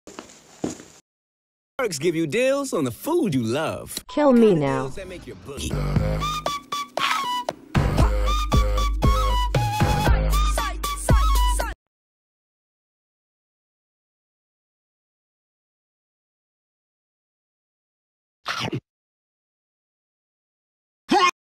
Grub hub ad but the grubhub guy hates the food
I was litteraly grossed out making this video due to how bad grubhub is lol